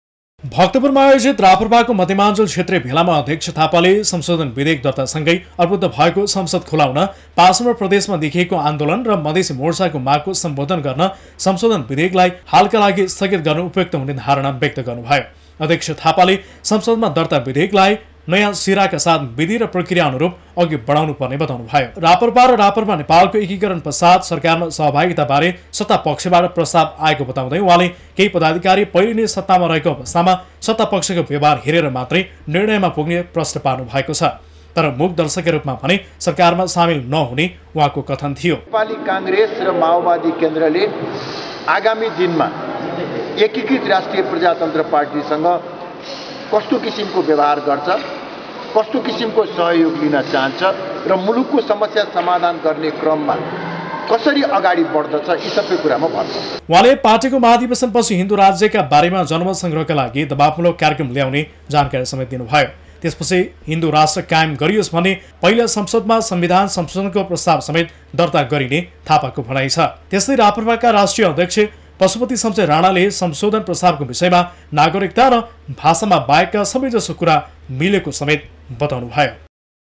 संसदलाई उपयोग गर्दै अघि बढ्न एमालेलाई कमल थापाको सुझाव - NEWS24 TV
राप्रपाका अध्यक्ष कमल थापाले व्यवस्थापिका संसदको अवरोध हटाउन नयाँ शिराबाट प्रक्रिया थालनी गर्नुपर्ने बताउनुभएको छ । उहाँले संशोधनको विधेयक तत्कालका लागि स्थगित गरी अगाडी बढ्नु सबैका लागि श्रेयकर हुने बताउनुभयो ।